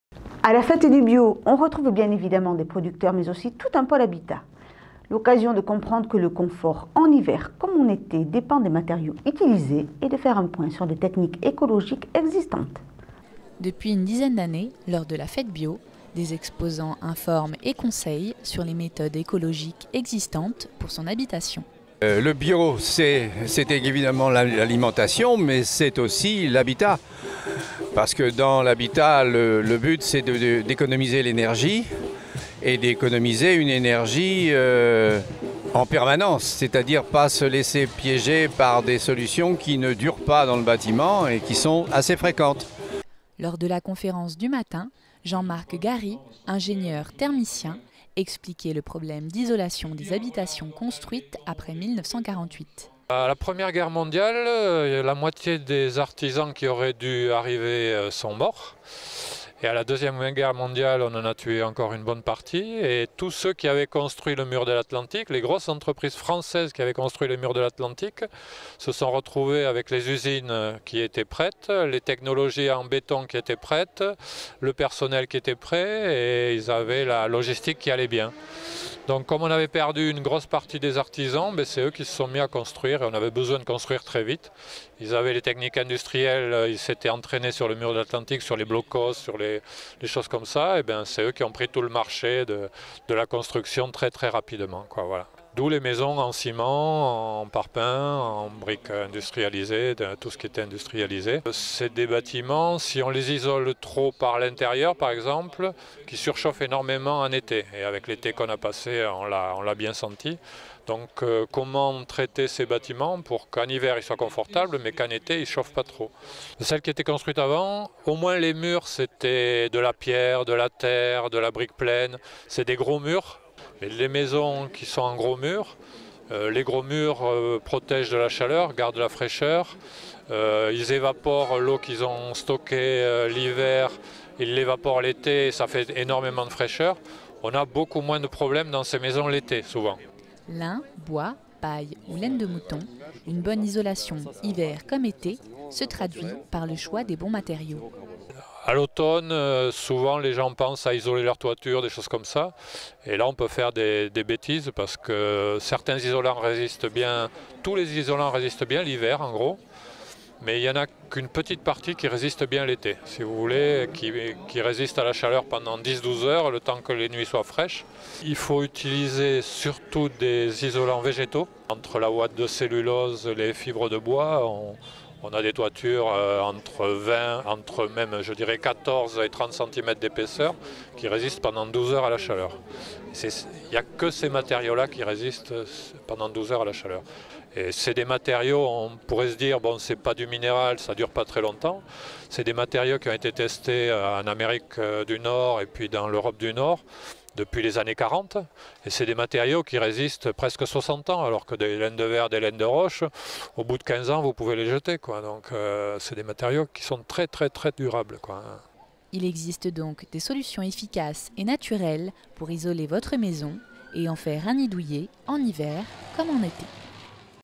Fête du bio à Labenne : le pôle habitat
A la fête du bio, on retrouve bien évidemment des producteurs mais aussi tout un pôle habitat. L'occasion de comprendre que le confort en hiver comme en été dépend des matériaux utilisés et de faire un point sur les techniques écologiques existantes.
MACS / Labenne, 5 Octobre 2015